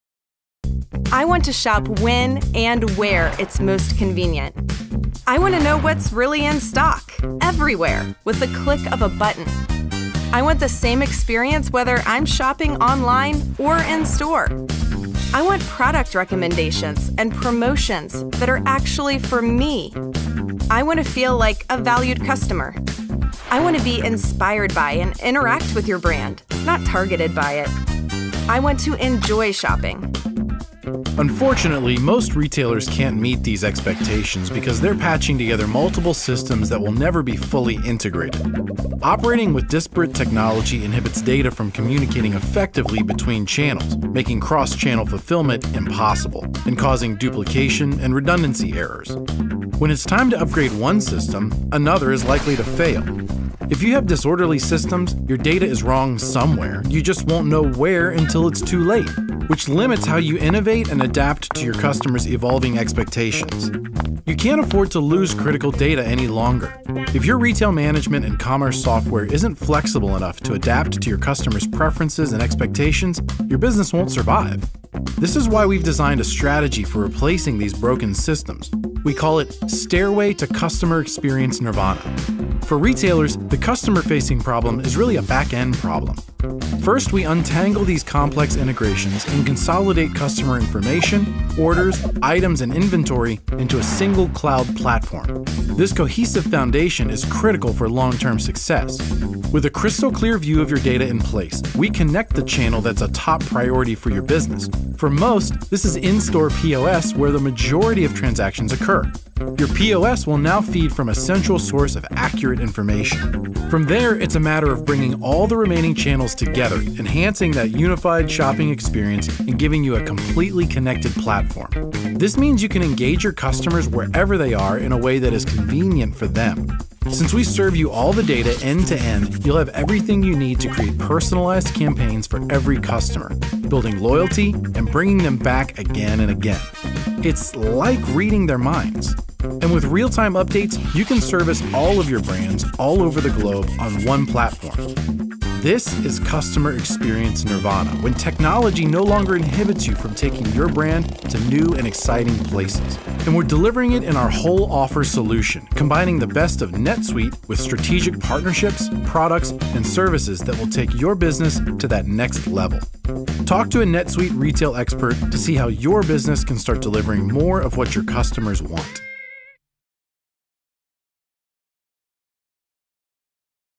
NetSuite para empresas de Moda | NetSuite for Fashion Brands
Las marcas de moda deben ofrecer a sus clientes una gran experiencia de compra, si quieren sobrevivir en el competitivo mercado actual.
NetSuite NetSuite ha ideado una estrategia con la que las marcas pueden eliminar la inhibición de la tecnología y poner en práctica una única plataforma de comercio unificado. Esto permite a las marcas de moda involucrar a sus clientes a través de diversos dispositivos en cualquier parte del mundo, en tiempo real.